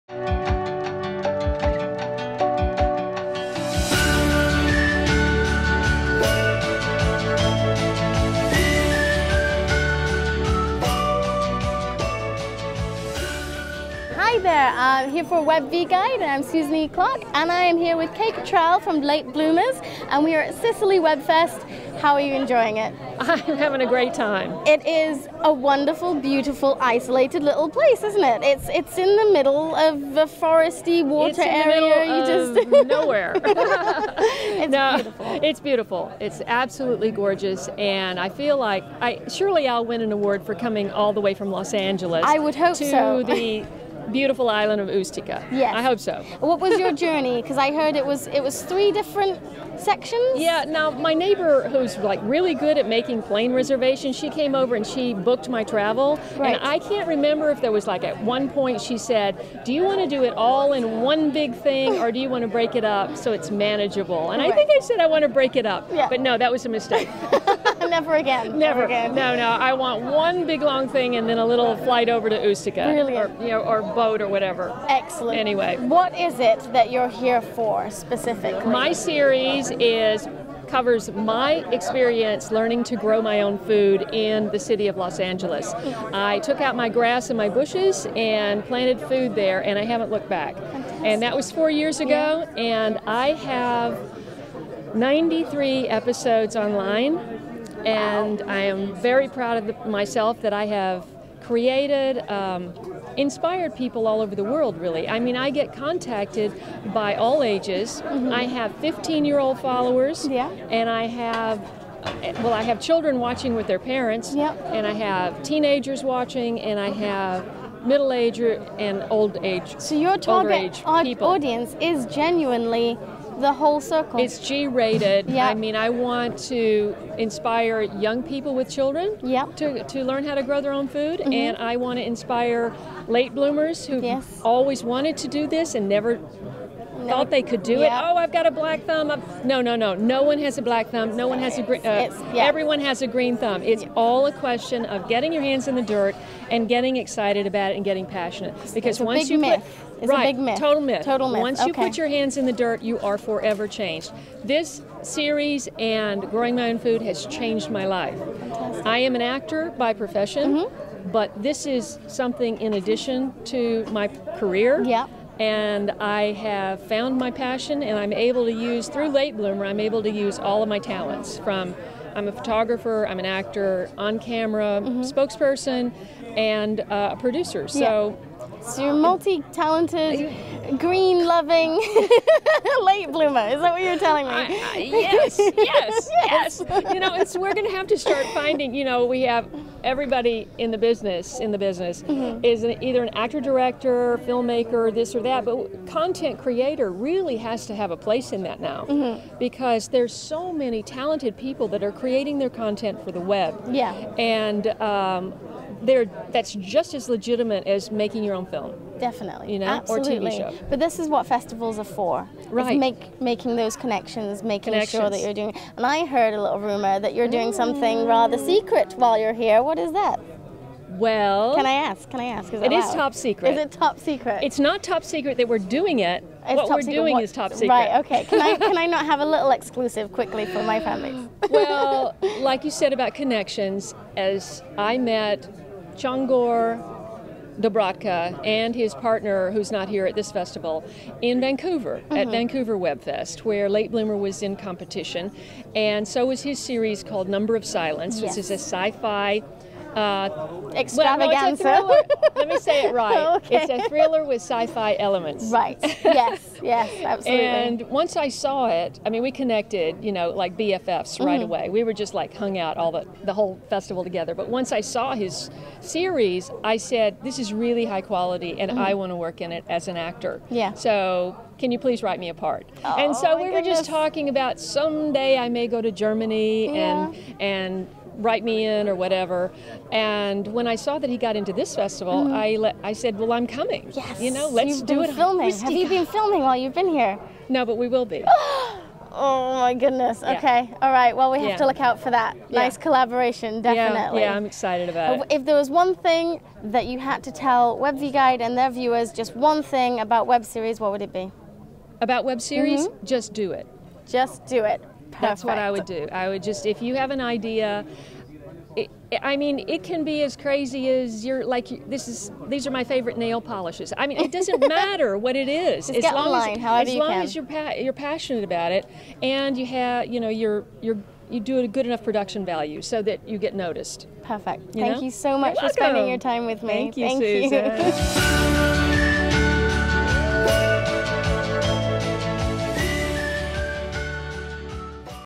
Susan E. Clarke interviews Kaye Kittrell, Creator of "Late Bloomer" at Sicily Web Fest 2016
Cinematographer/Editor Chris Rithin, Wolfpack Productions